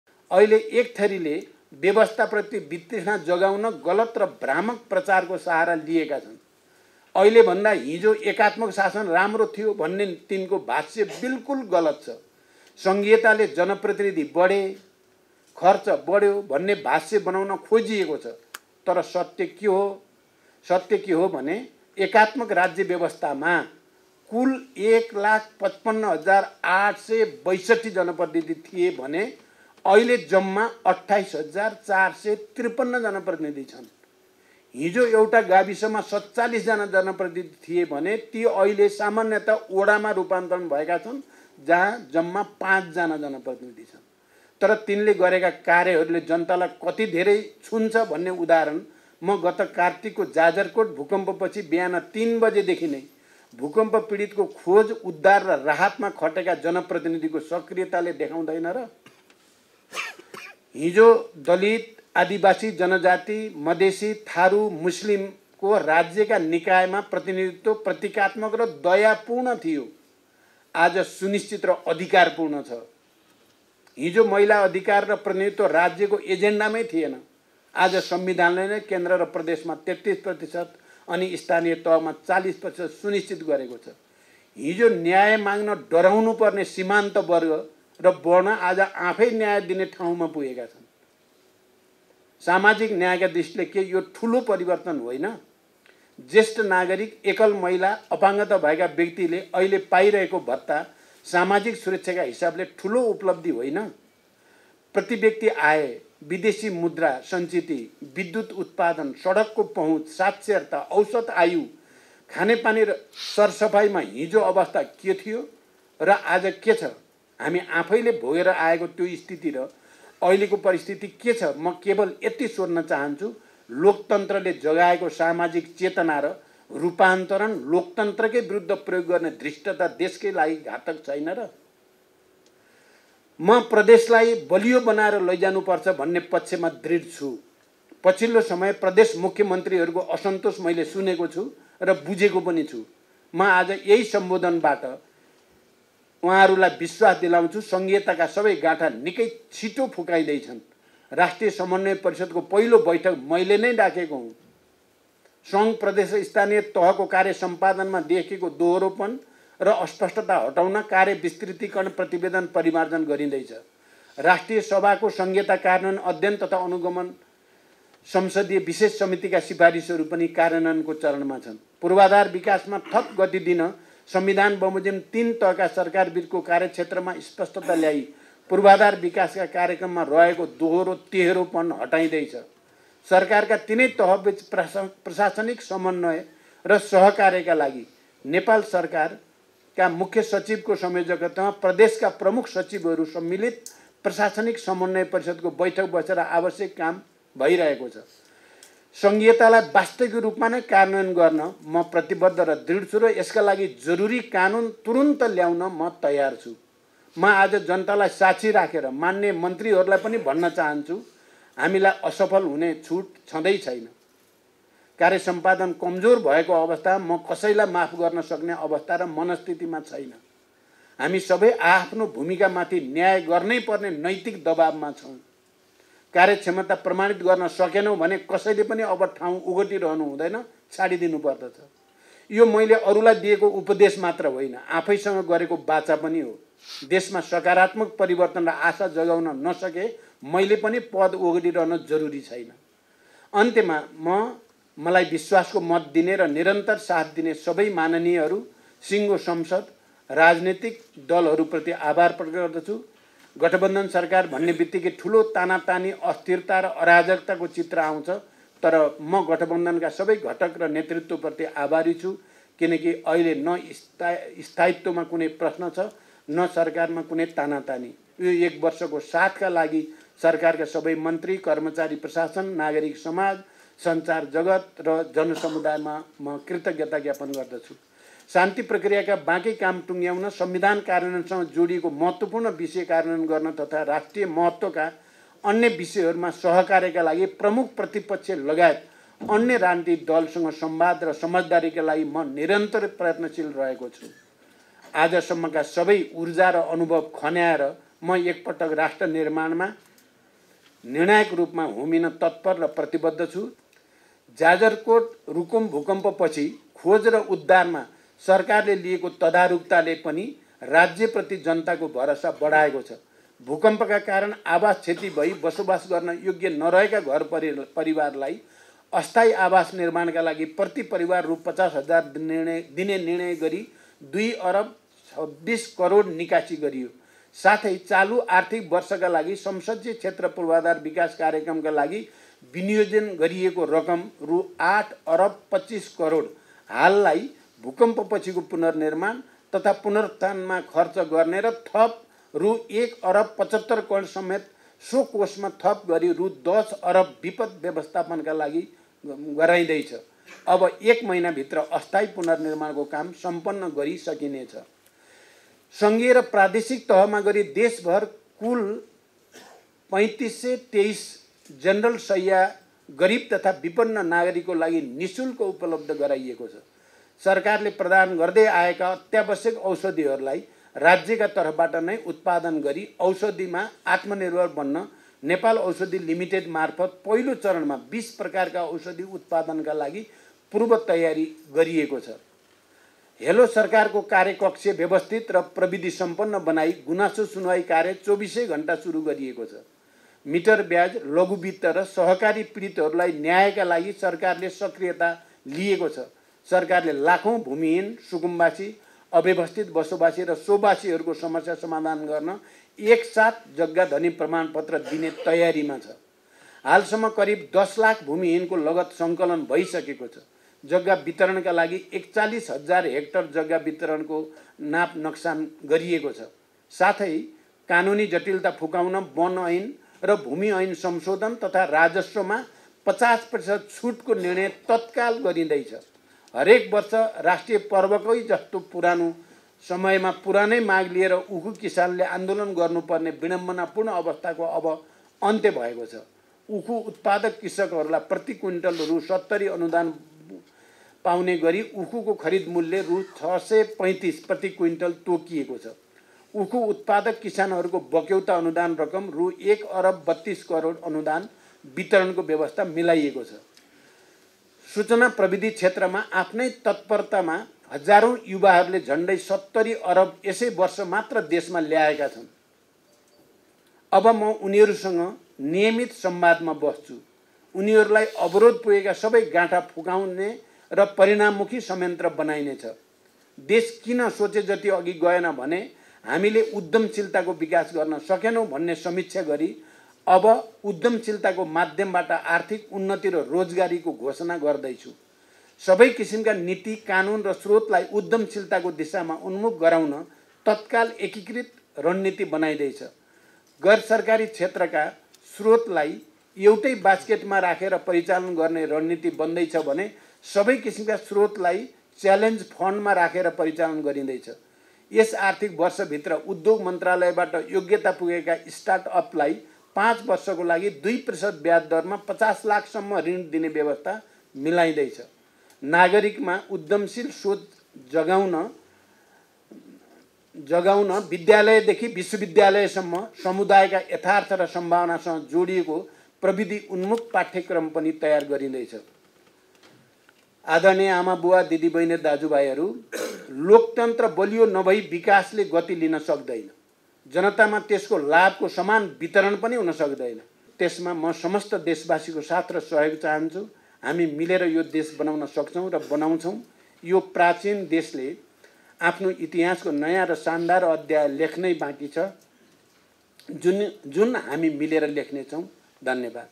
गणतन्त्रले केही भएन भन्नेलाई प्रधानमन्त्रीकाे जवाफ : संघीयताले खर्च कसरी बढायाे ?
Guest :
Camera/ Editing : Bhuwan K C || Anushg B C